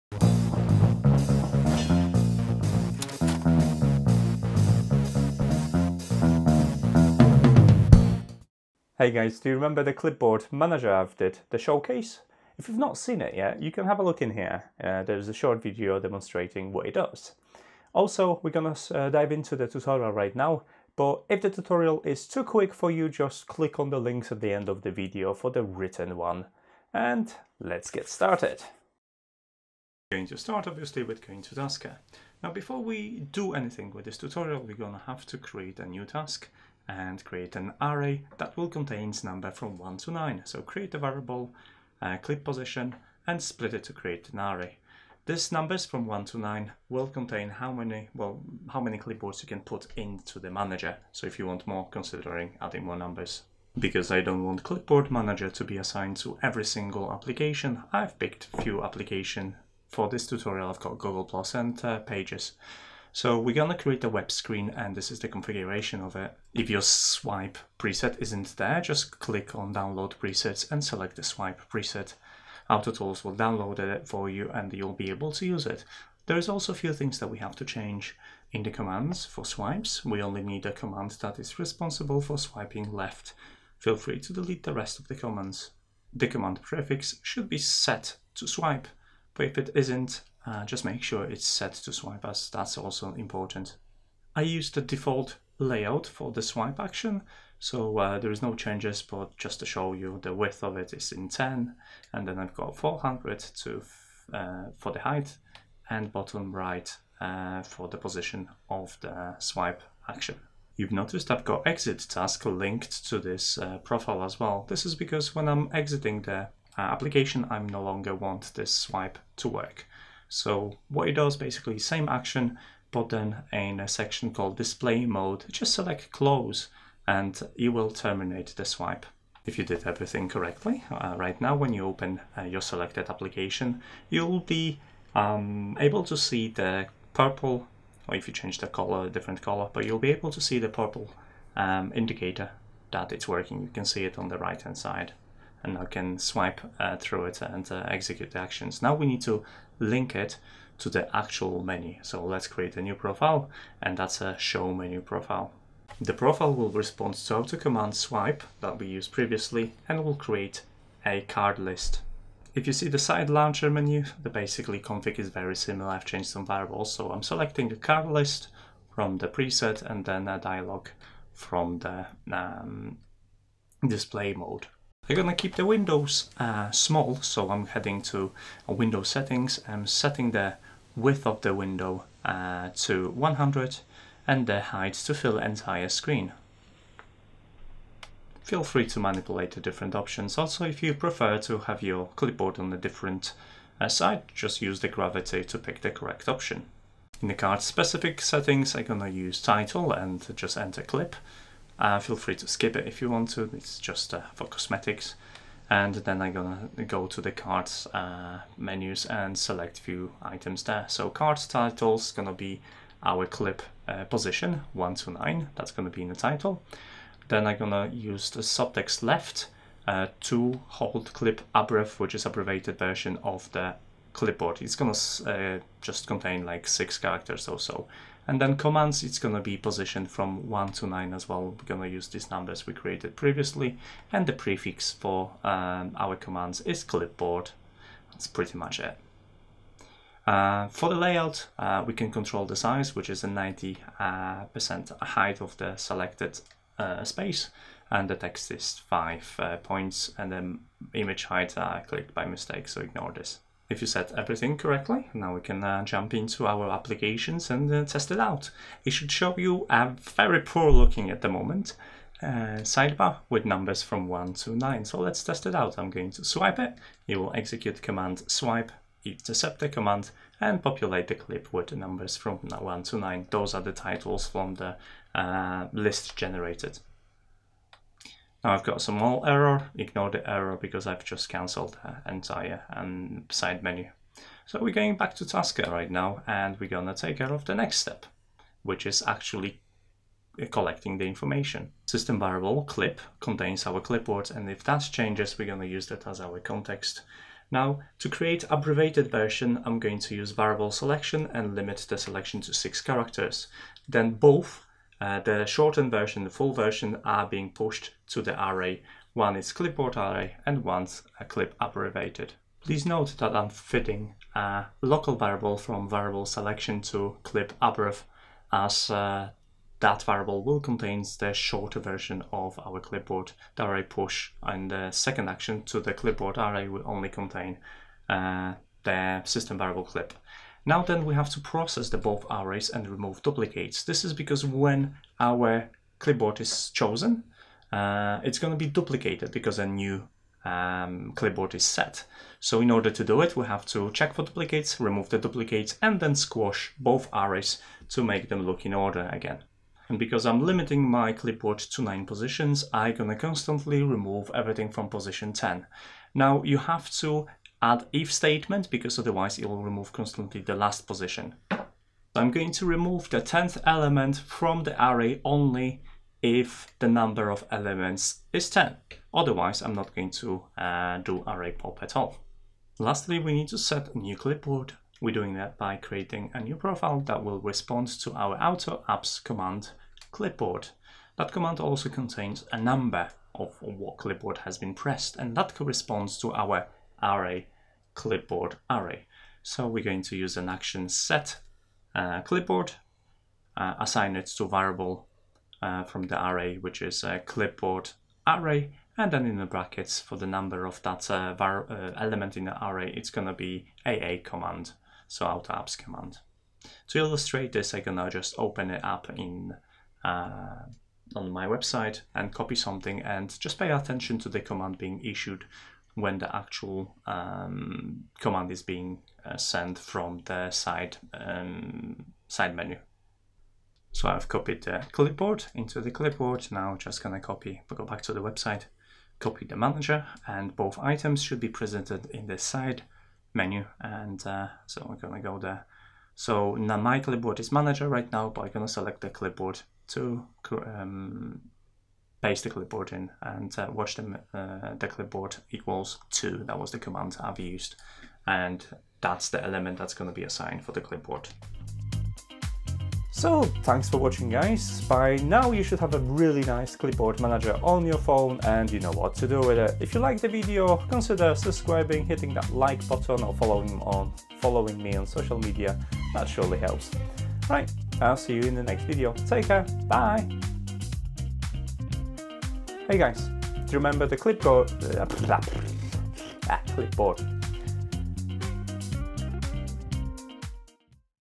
TASKER TUTORIAL - Clipboard Manager
INFO:
I'm creating a clipboard manager using Tasker and AutoTools and it's awesome!

Used in this tutorial:
AutoTools: